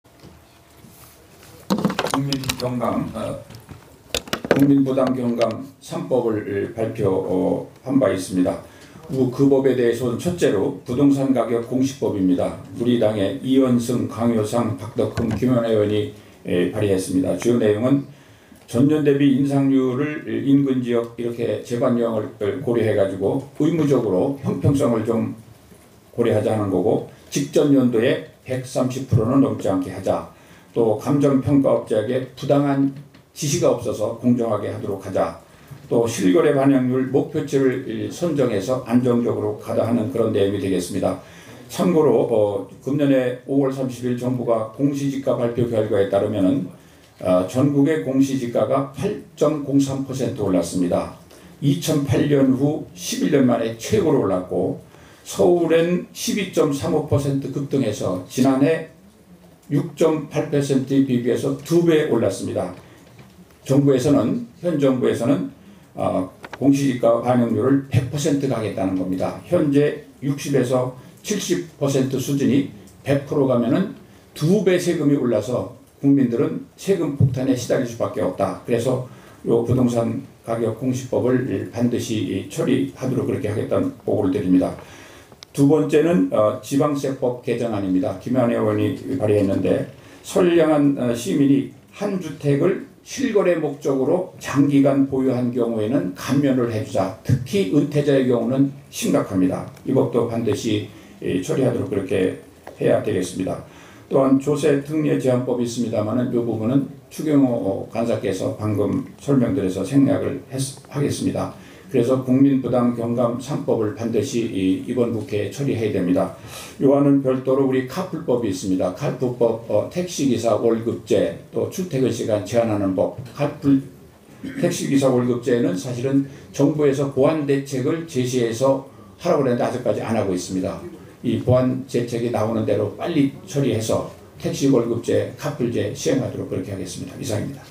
자유한국당 이현재, "문정권, 공시지가 100% 적용 2배 세금 올려 국민들 세금 폭탄에 시달리게 돼..."
자유한국당 이현재 의원은 19일 열린 정책의원총회에서 문정권이 공시지가  100%적용은  국민들이 세금폭탄에 시달리게 되는 정책이라고 말했다.